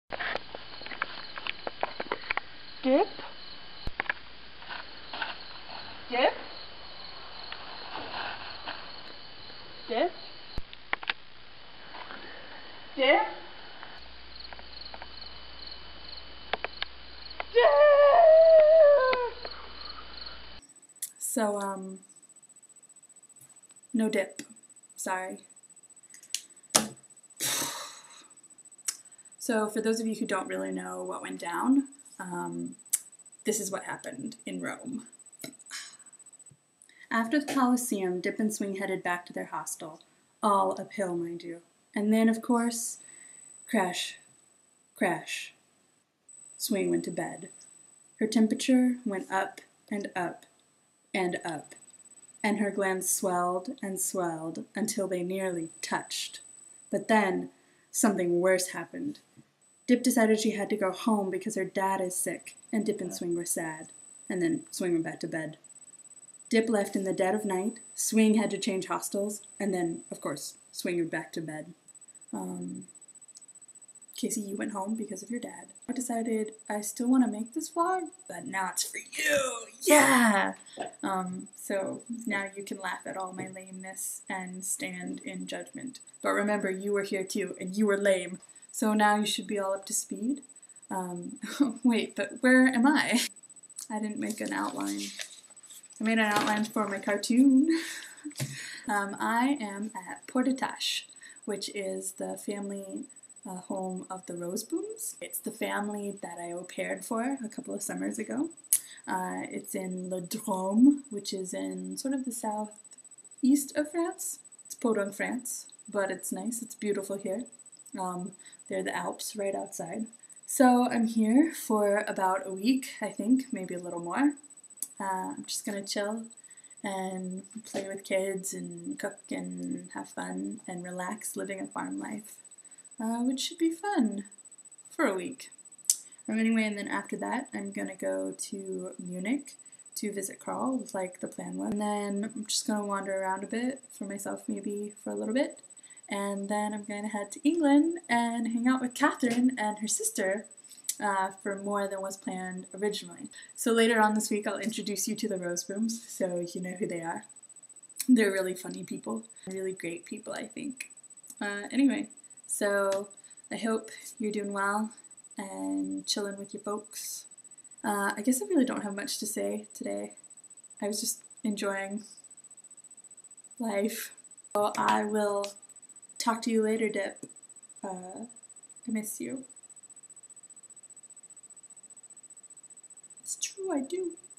pouting and Pordatach
Swing gets into the  swing of this life sans Dip, and changes it up a bit too. It's semi-awesome

(oh and yeah, Casey, that's your pen I'm bandying about through most of the video... you can MAYBE have it back when I return.)